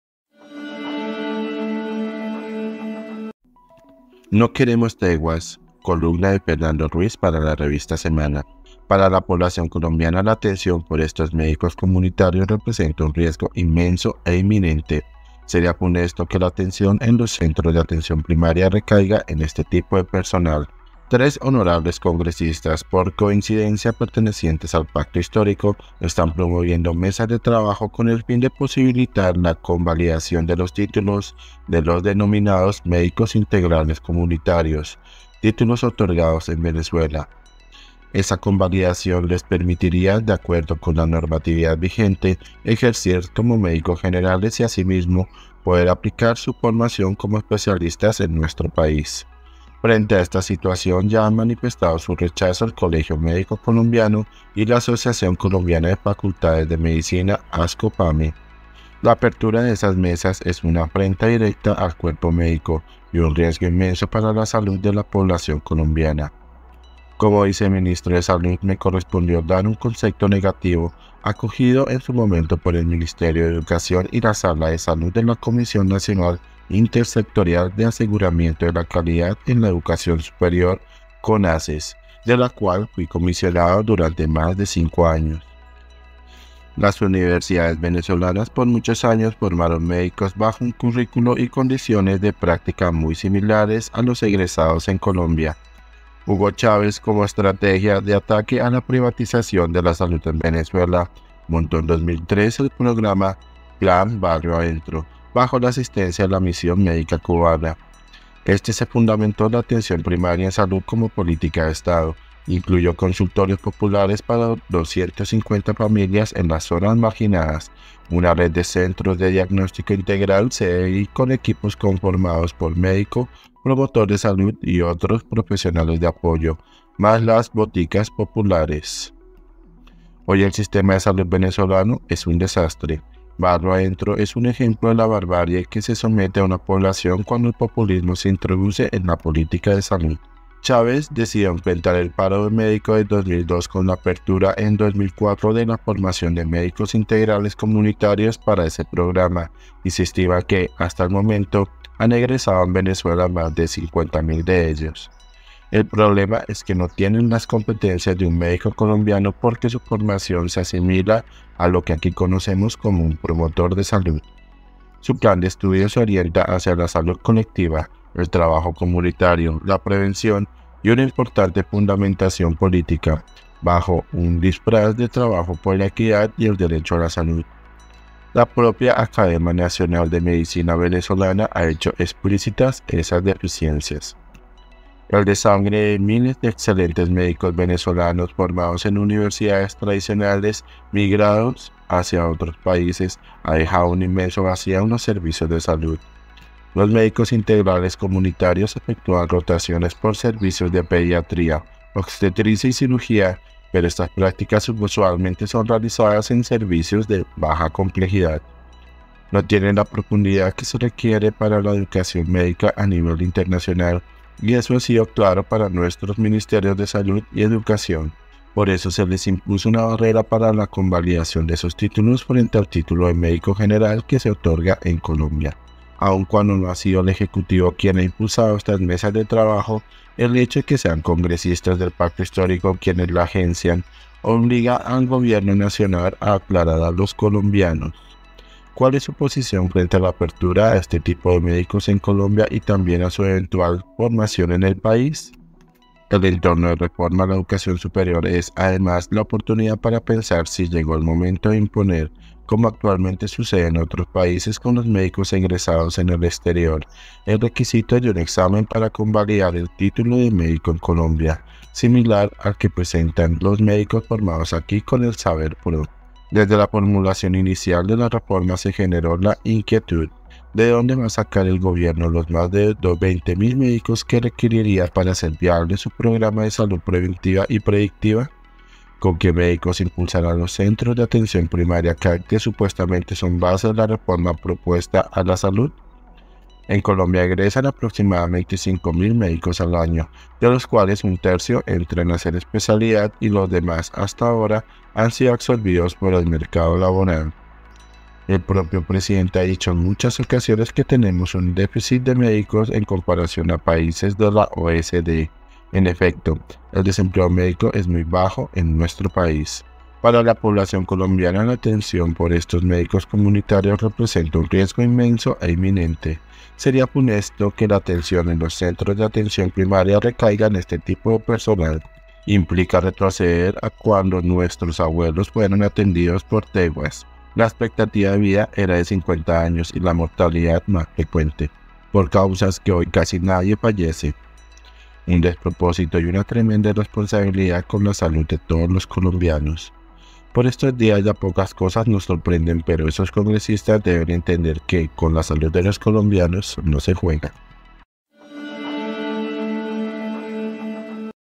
NO QUEREMOS TEGUAS 🛑 Por Fernando Ruiz
Para la población colombiana la atención por estos médicos comunitarios representa un riesgo inmenso e inminente. Sería funesto que la atención en los Centros de Atención Primaria recaiga en este tipo de personal.
columna Fernando Ruiz | Revista Semana